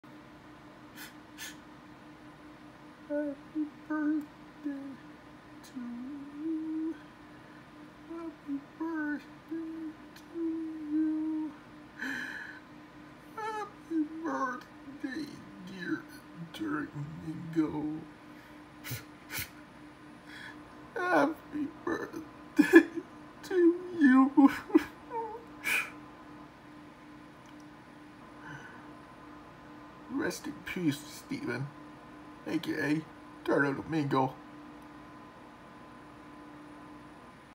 I was singing sad birthday on Steven aka Turtlemonade
Sad Credit to Steven aka Turtleamigo

He’s was harm himself what happened early

Rest In Peace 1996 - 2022